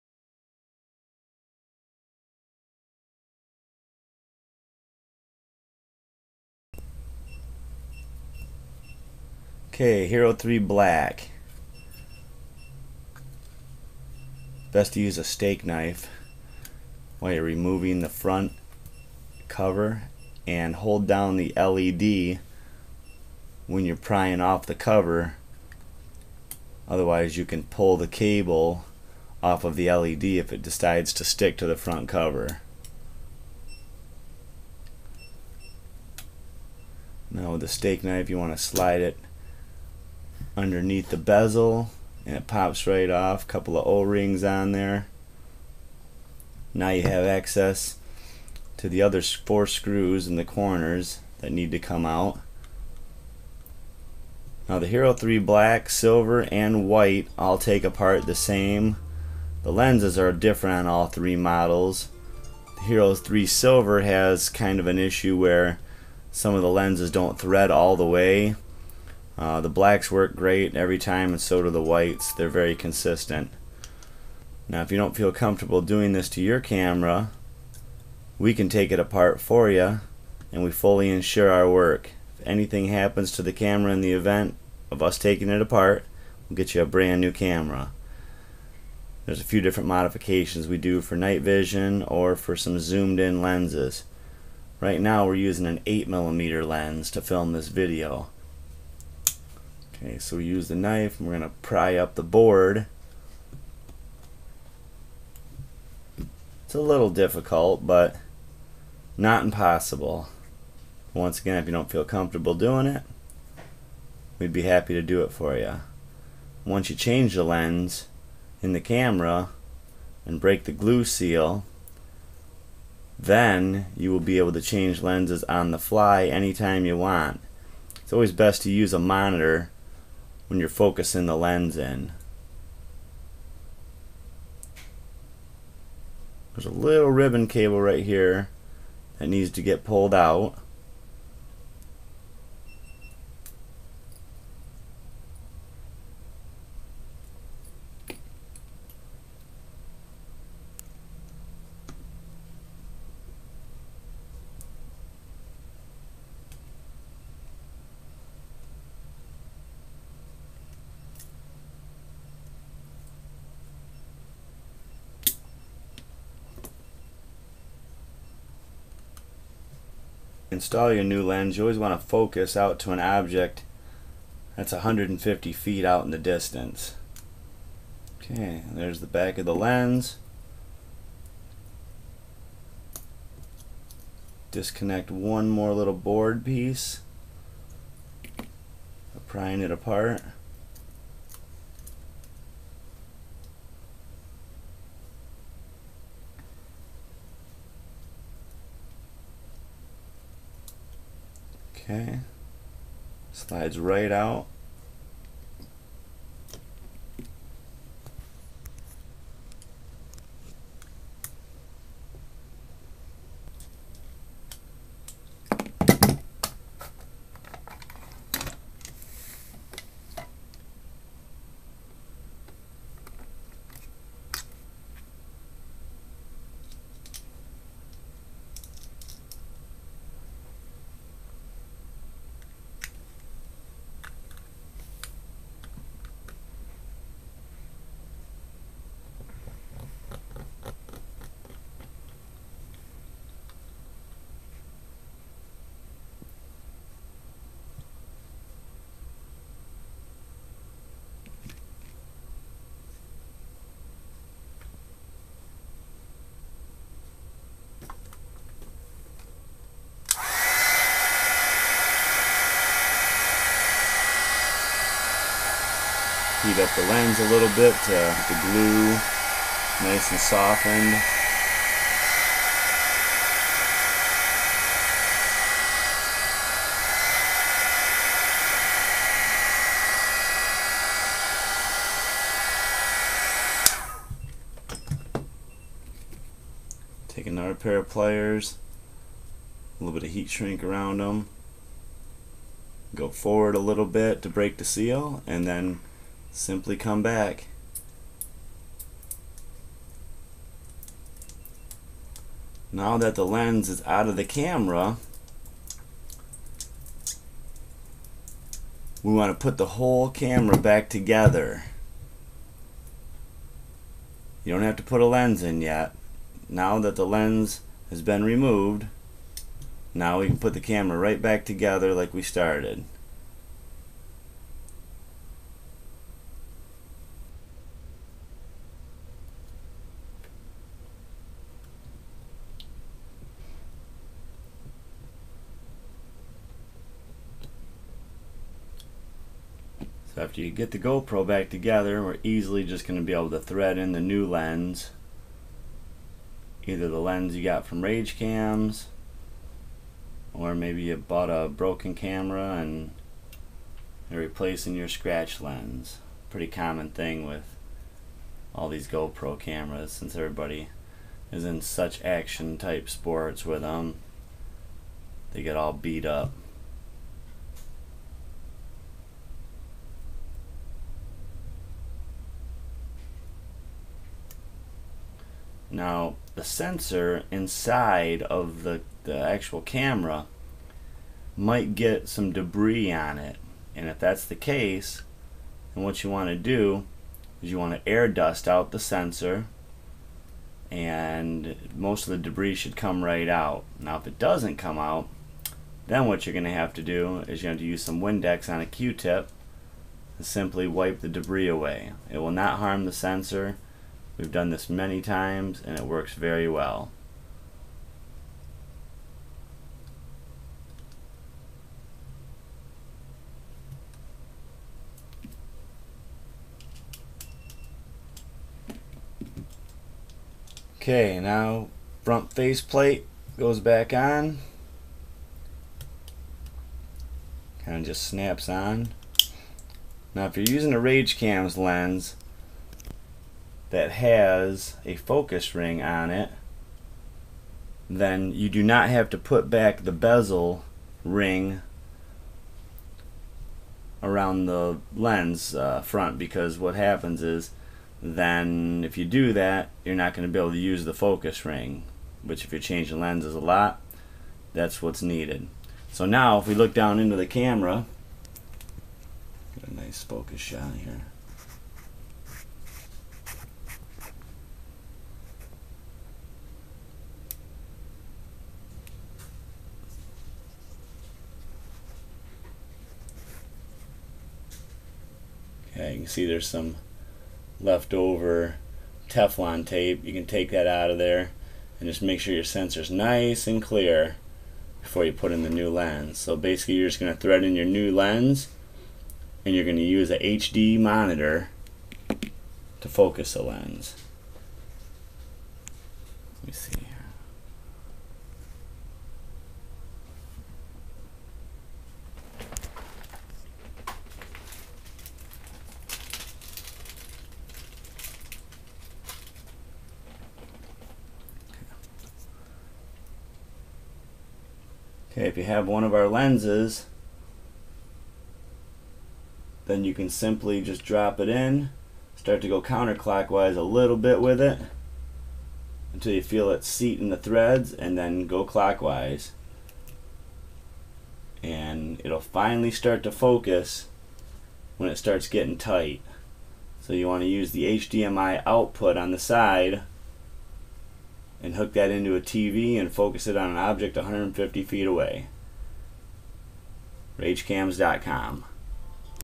Gopro Hero3 Lens Change Installation Instructions Black/Silver/White RageCams Hero 3 Scratched Lens
**Narrated by RageCams Owner - **Mod done by RageCams Female Tech :)

How to change your GoPro HD Hero3 lens to a flat no fish-eye view.
(Same for Black, Silver & White, with minor differences.)
You will need the following tools:
1- t4 tool
1- Phillips screwdriver (Used more for the HERO3 PLUS EDITIONS)
2- pliers 1 coated with black tape or heatshrink
1- steak knife (or flat edge tool, our steak knife is quite dull, but provides the leverage to pop off the bezel.)
1- heat gun or hair dryer
1- air duster, qtip & windex (if debris gets on the sensor)
1- steady hand